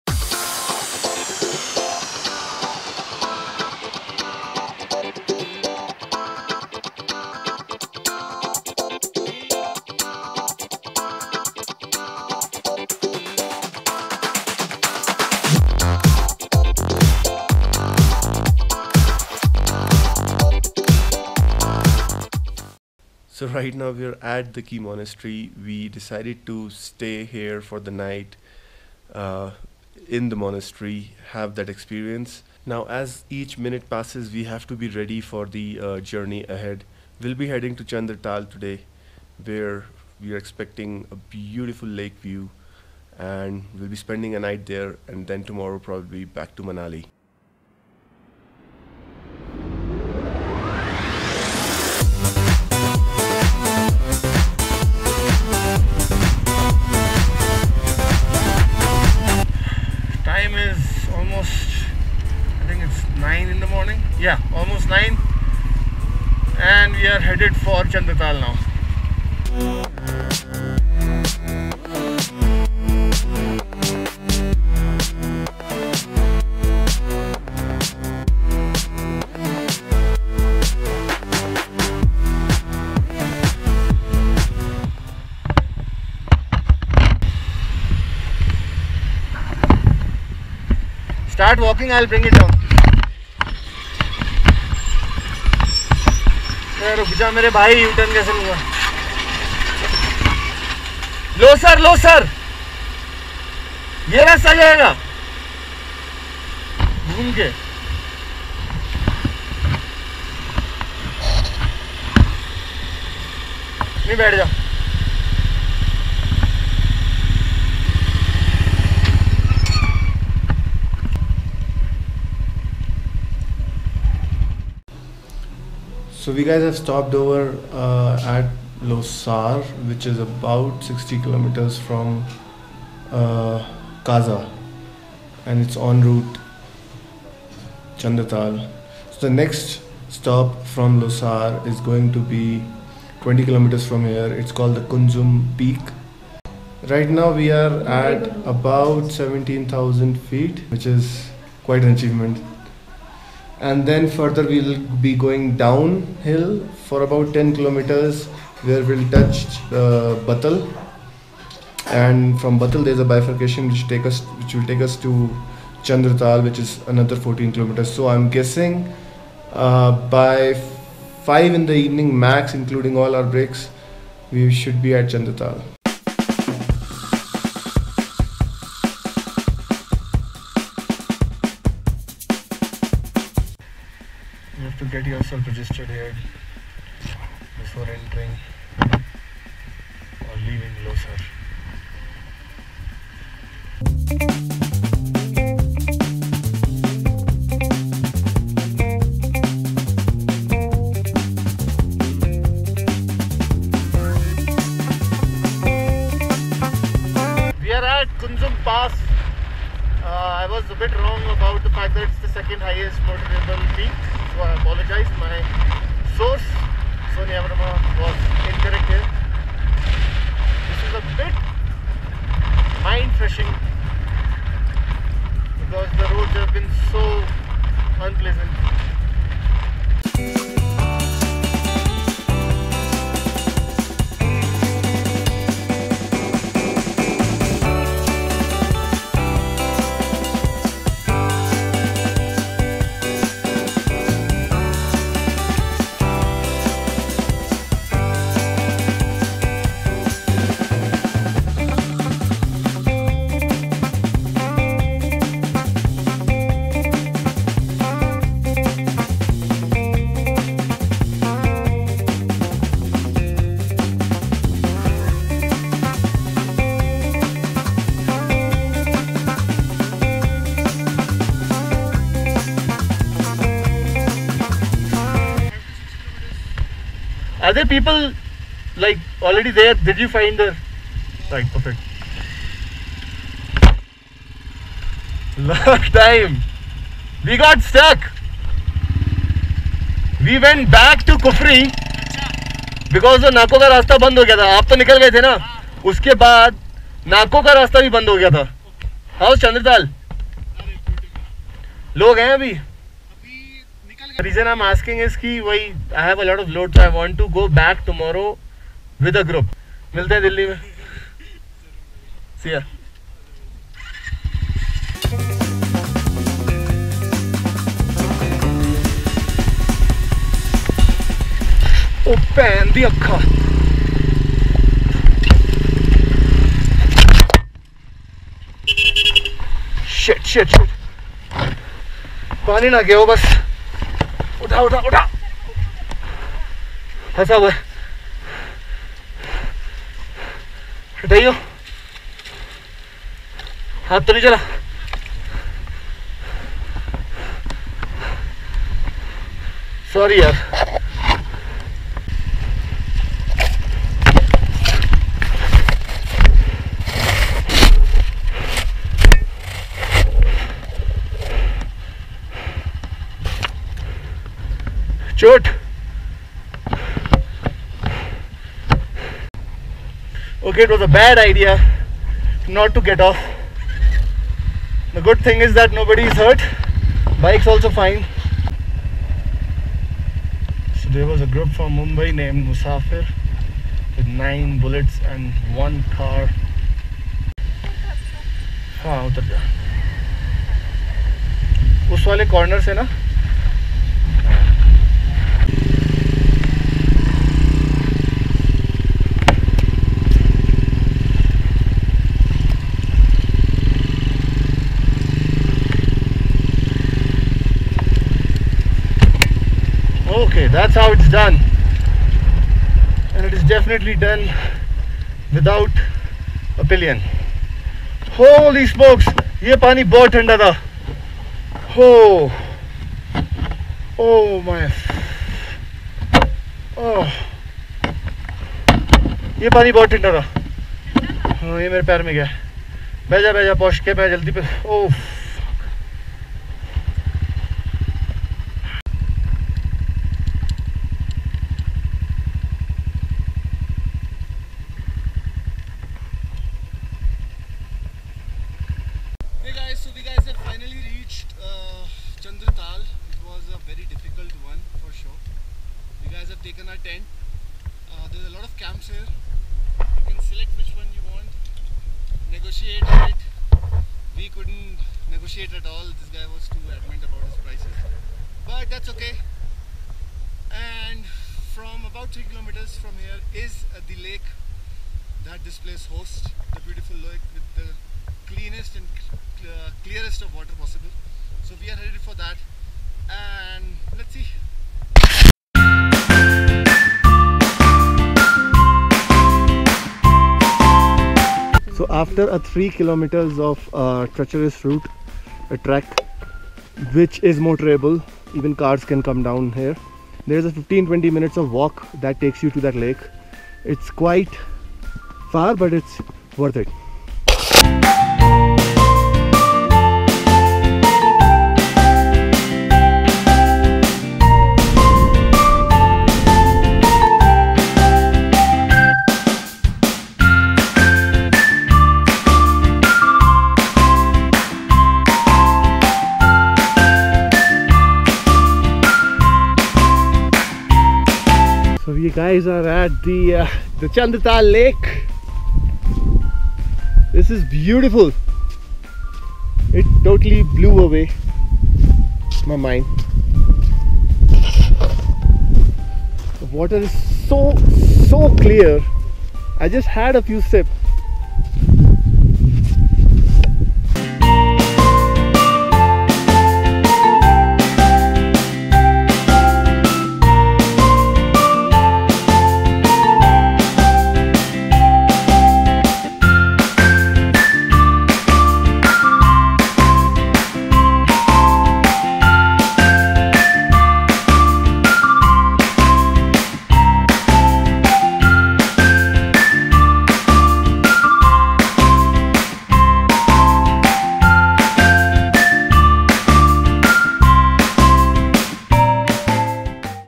Kaza to Chandratal - The Mesmerizing Lake | Tour Of Spiti EP05
Chandra Tal is situated in the Spiti part of the Lahul and Spiti district of Himachal Pradesh. The name of the lake originates from its crescent shape. It is situated at an altitude of about 4,300 metres in the Himalayas